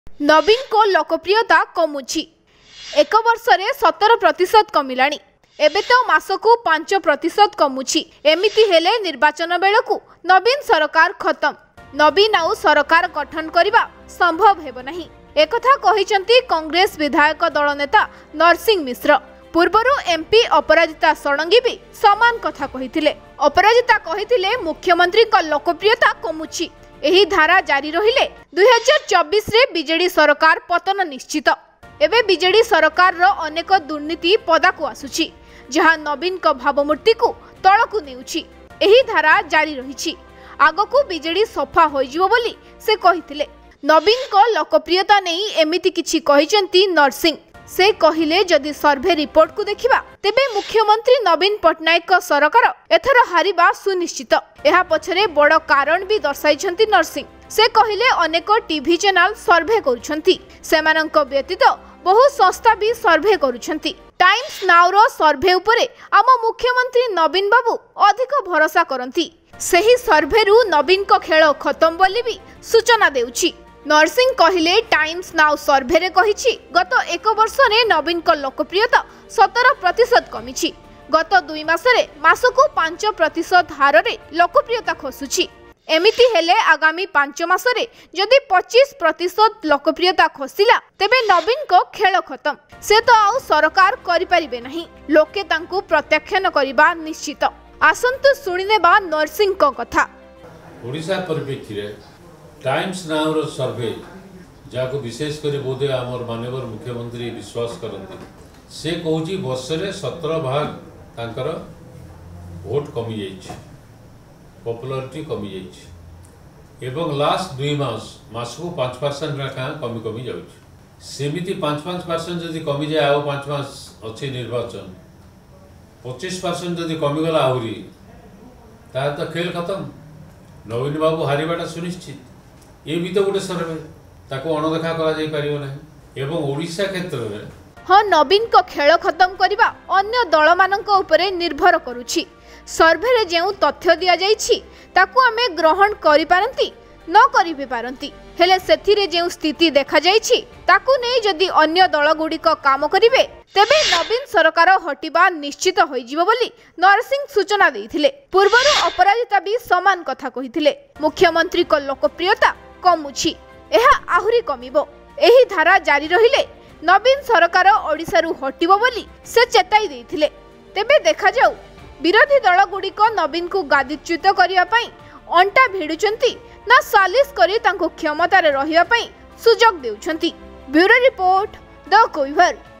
ନବୀନ ହାରିବା ସୁନିଶ୍ଚିତ | Naveen's defeat destined | THE Quiver
ନବୀନଙ୍କ ଲୋକପ୍ରିୟତା କମୁଛି । ୧ବର୍ଷରେ ୧୭ ପ୍ରତିଶତ କମିଲାଣି । ଏବେ ତ ମାସକୁ ୫ ପ୍ରତିଶତ କମୁଛି । ଏମିତି ହେଲେ ନିର୍ବାଚନ ବେଳକୁ ନବୀନ ସରକାର ଖତମ । ନବୀନ ଆଉ ସରକାର ଗଠନ ସମ୍ଭବ ହେବନାହିଁ । ଏକଥା କହିଛନ୍ତି କଂଗ୍ରେସ ବିଧାୟକ ଦଳ ନେତା ନରସିଂହ ମିଶ୍ର ।

Naveen's popularity is decreasing. 17 percent of popularity has reduced in last 1 year. Now it is decreasing by 5 percent per month. In this case, Naveen's popularity will completely lost by the time of the election. Naveen will no longer be able to form  the government said Congress Leader Narsingh Mishra.